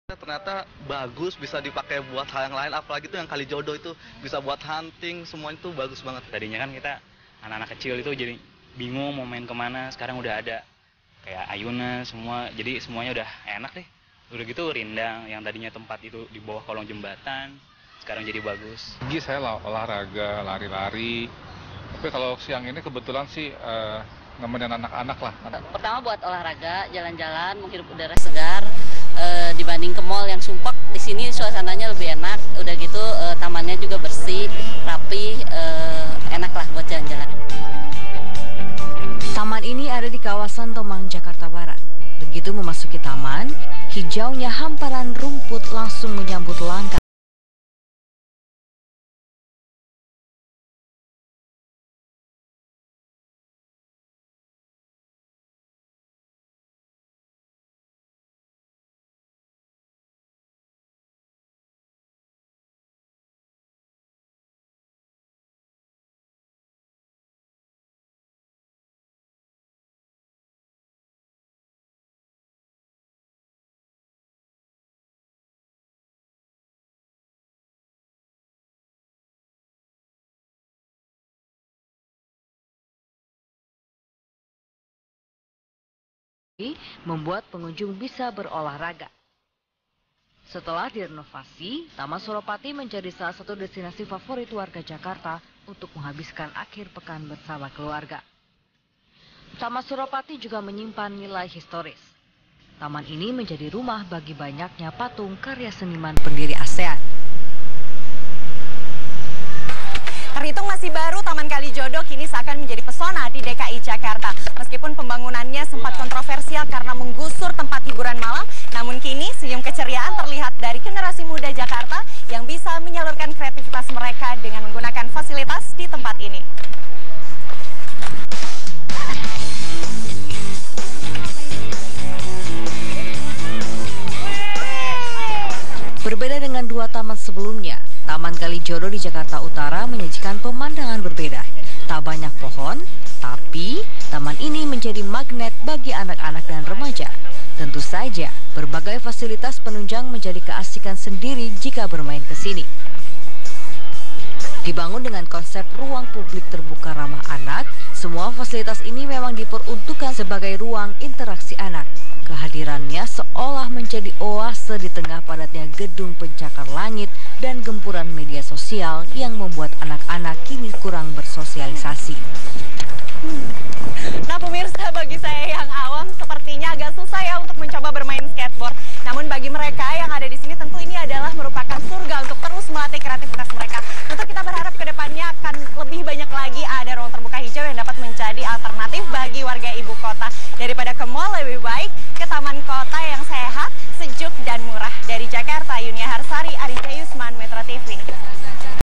Kumpulan video hasil kerja Ahok terbaru 4B. Taman Cattleya, Surapati dan Kalijodo
Kumpulan video hasil kerja Ahok-Jarot dan Pemprov DKI terbaru. Penataan taman cantik taman Cattleya, taman Seropati dan Taman Kalijodo terbaru.